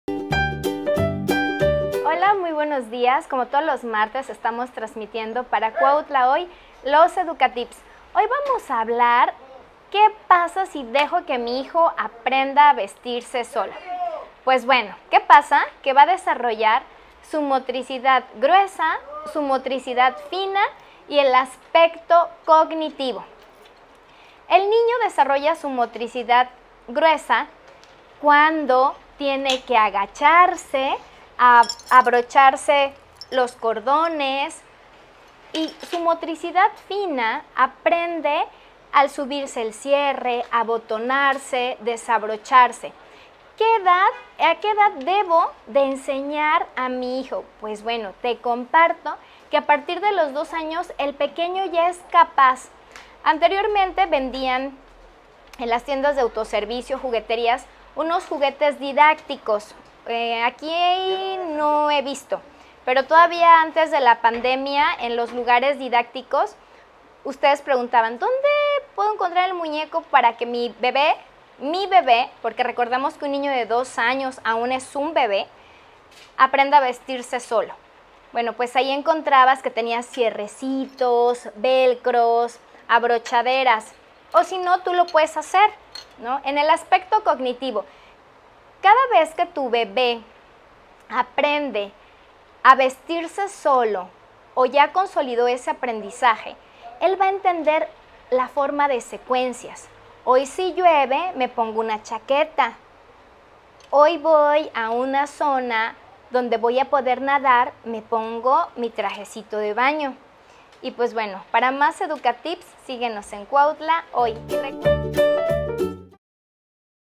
¿Qué pasa si dejas a tu hijo aprenda a vestirse solo?.
Decerem Estrada Vazquez nuestra experta en pedagogía, nos explica sobre ¿Qué pasa si dejas a tu hijo aprenda a vestirse solo?.